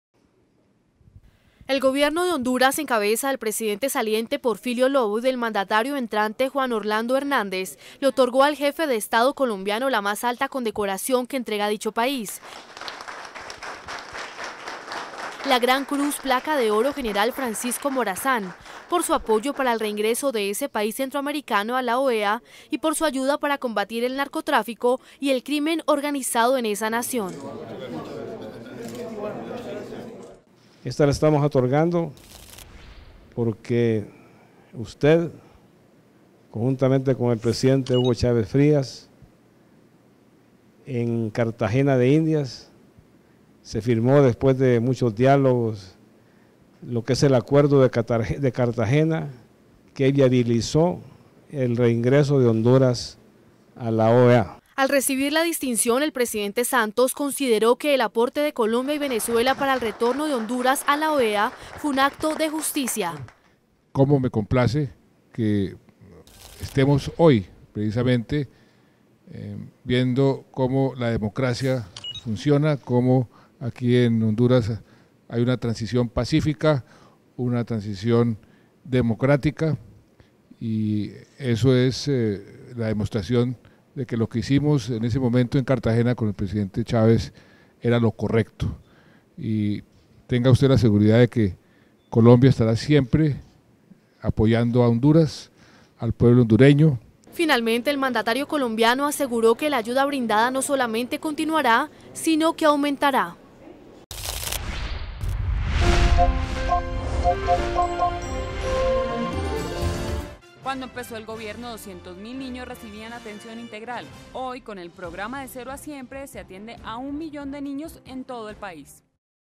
Gobierno Hondureño condecoró al Presidente Juan Manuel Santos
Gobierno Hondureño condecoró al Presidente Juan Manuel Santos - 2 de febrero de 2014